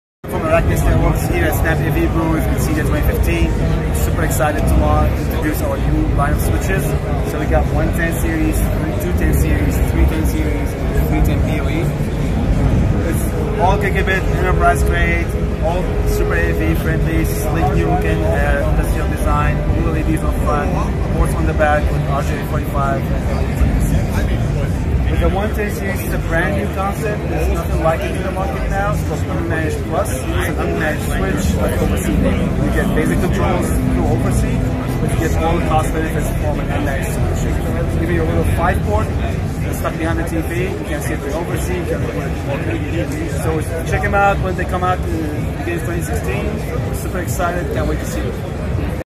CEDIA Expo 2015: New 110, 210, and 310 Switches from Araknis Networks
Coming soon to the Araknis Networks line of switches - 110, 210, and 310 models. We caught up with Category Manager, Ayham Ereksousi at CEDIA Expo 2015 to get the details.